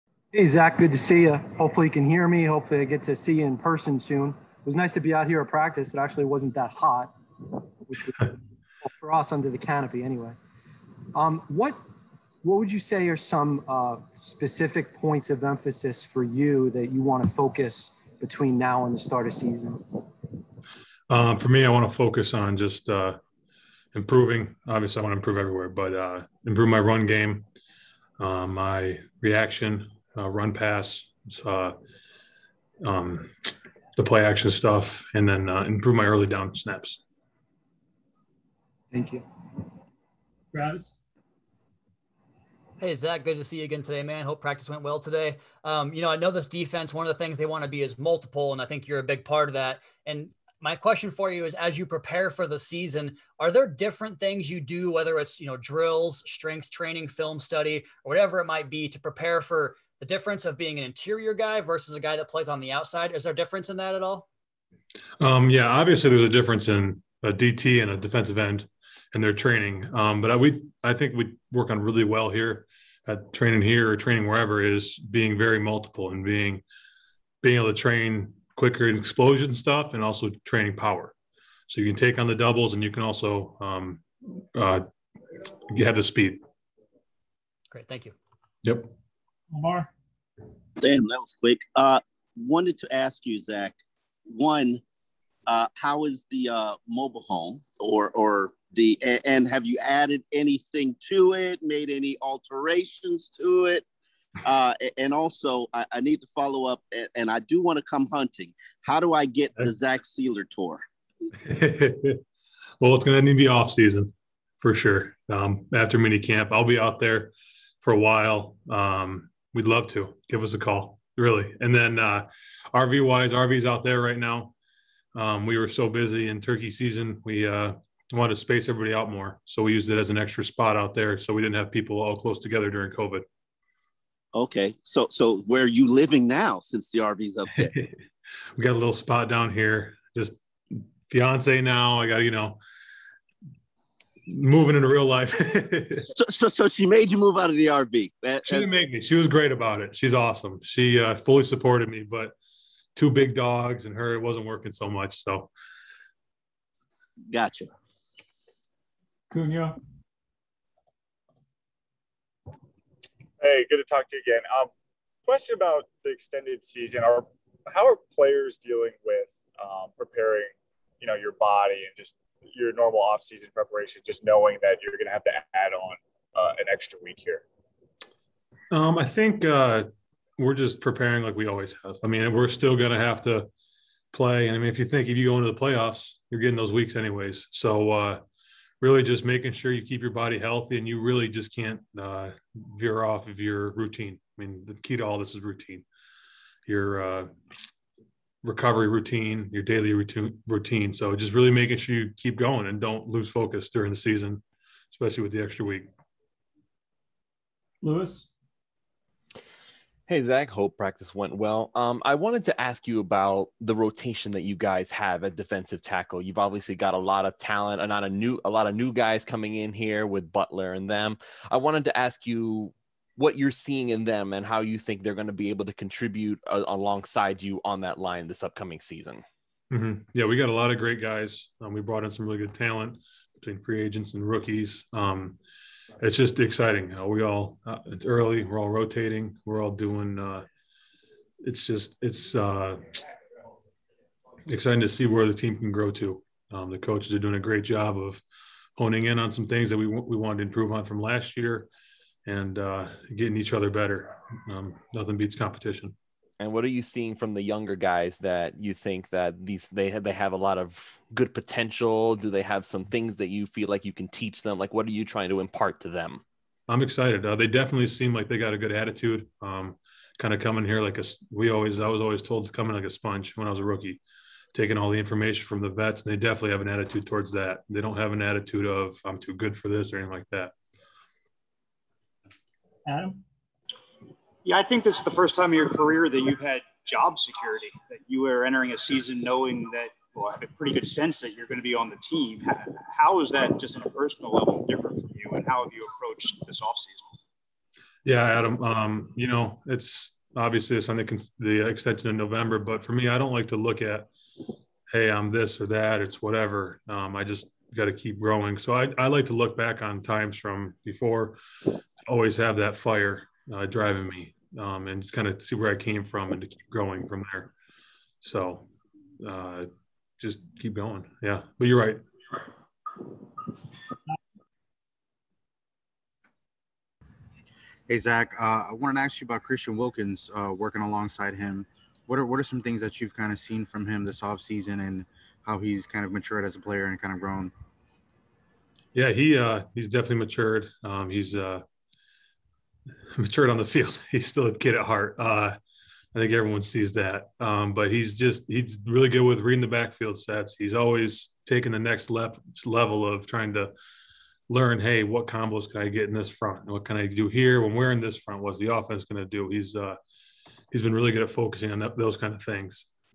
Zach Sieler on the Start of OTAs | Miami Dolphins Media Avails
Zach Sieler meets with the media on May 26, 2021 to discuss the start of OTAs.